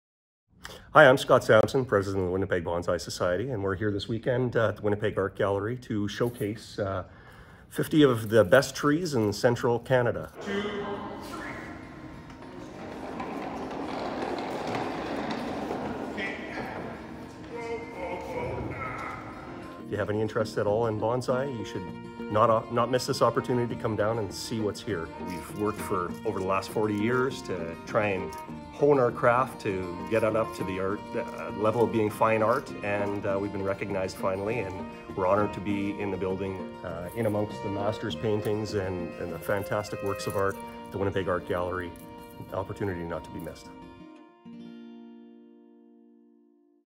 Bonsai trees sprouting up at the Winnipeg Art Gallery
If you pass the WAG-Quamajug today, you might notice dozens of tiny trees through the windows.
Those are called Bonsai trees.
It's the Japanese art of growing and training miniature trees in containers.
And it's a collaboration Bonsai Society of Winnipeg president Scott Samson says he's been working on for decades.
CBC's Jim Agapito had a chance to speak to him at the WAG exhibit.